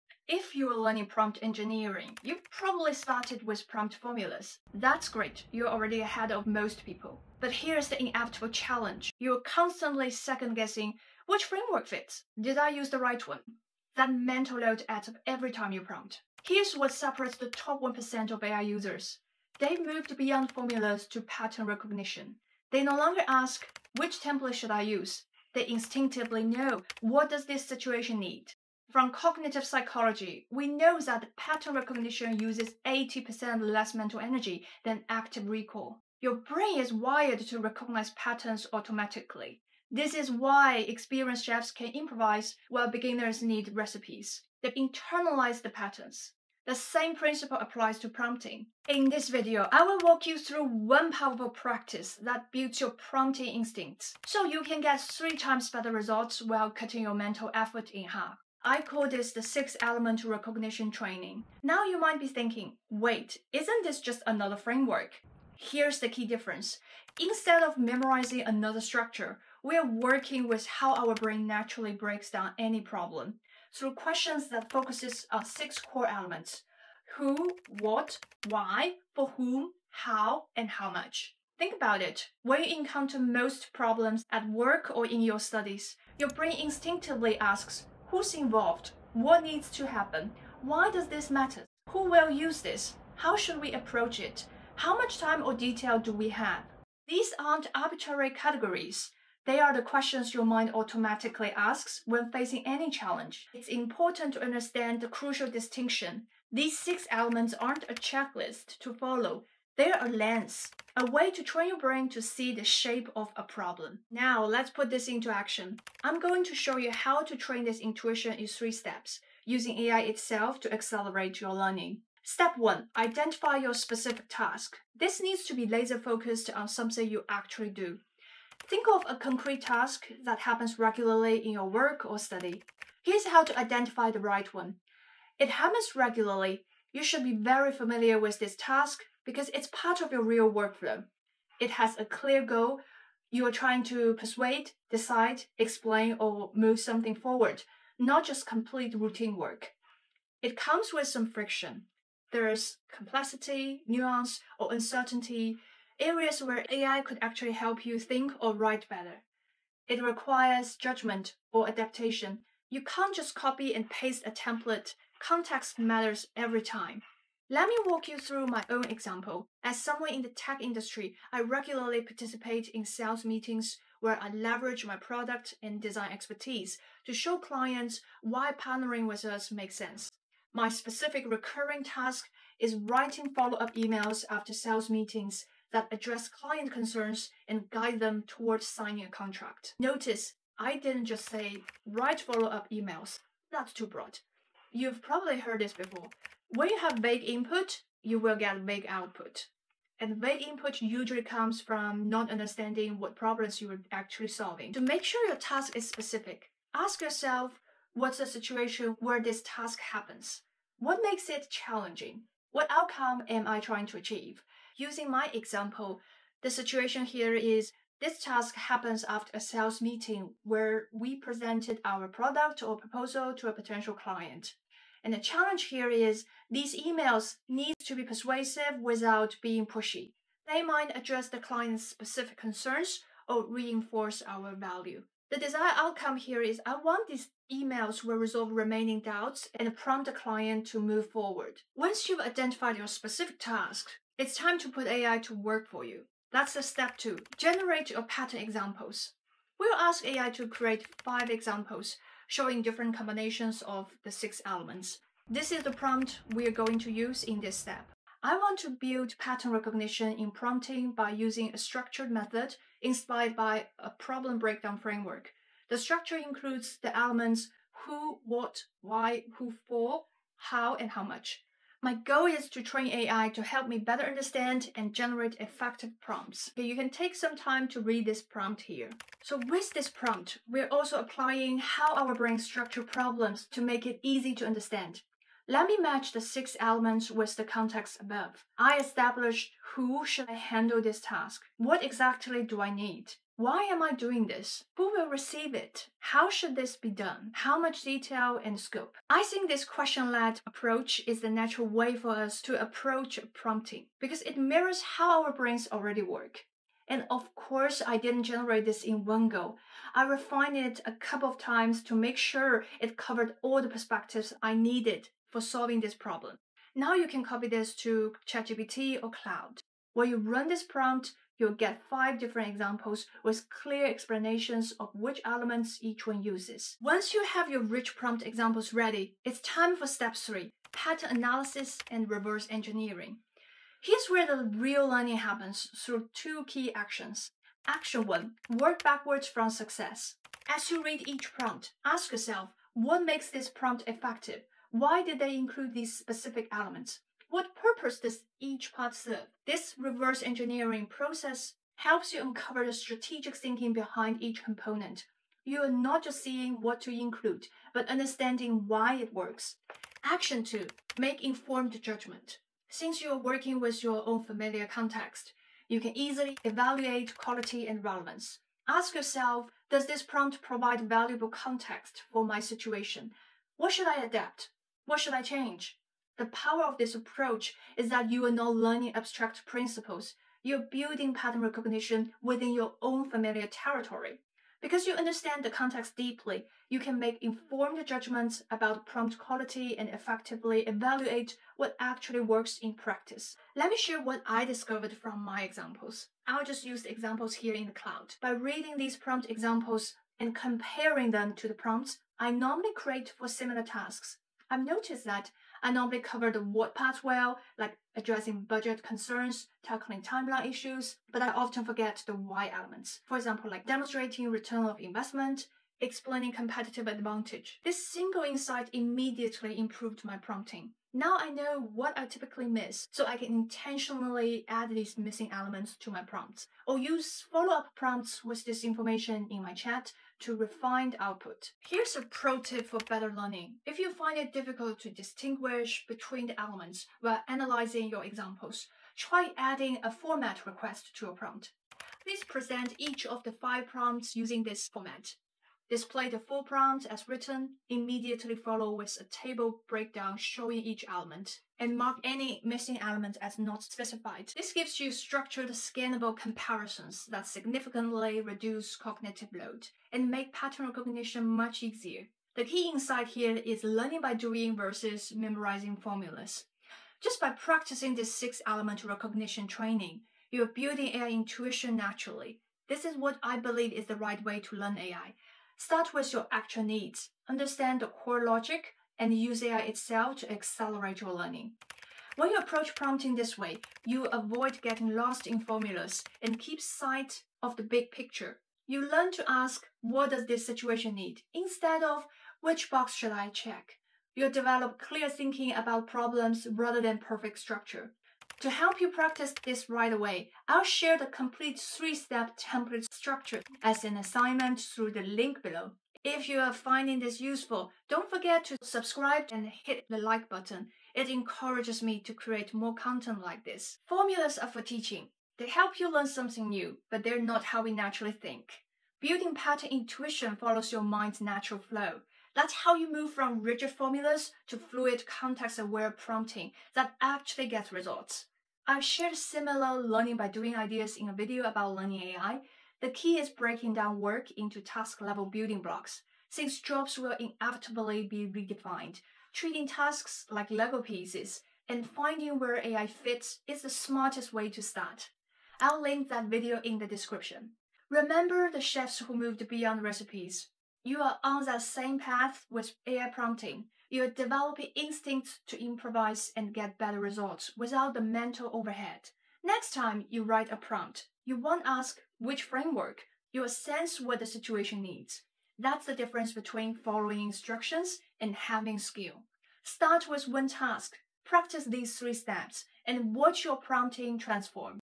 ChatGPT Prompt Engineering: What Top 1% Users Do (Beyond Basic Formulas)
Stop memorising prompt formulas and start building AI instincts like the top 1% of users. This comprehensive guide reveals the 6-Element Recognition Training method that cuts your mental effort in half while delivering 3x better ChatGPT results.

If you've ever felt stuck trying to figure out which prompting approach to use in real-life situations, this pattern recognition method will change the game. Learn the cognitive psychology behind why experienced AI users succeed — and how to develop those same intuitive skills yourself.

Perfect for anyone who wants to learn, level up, and master AI tools like ChatGPT and Claude — no matter your background, skillset, or experience level.

📋 Get the complete 3-step assignment with all prompts used in this video

💡 Interested in more "learning by doing" AI strategies? Check out my video on breaking work into task-level building blocks

**Timeline:**
0:00 - Why prompt formulas create mental overload
0:18 - The problem breakdown approach top 1% users know
2:03 - Step 1: Identifying your specific task
4:06 - Step 2: Generate pattern examples with AI
5:30 - Step 3: Pattern analysis and reverse engineering
7:11 - Pro tip: Format requests for better learning
8:33 - From rigid formulas to fluid thinking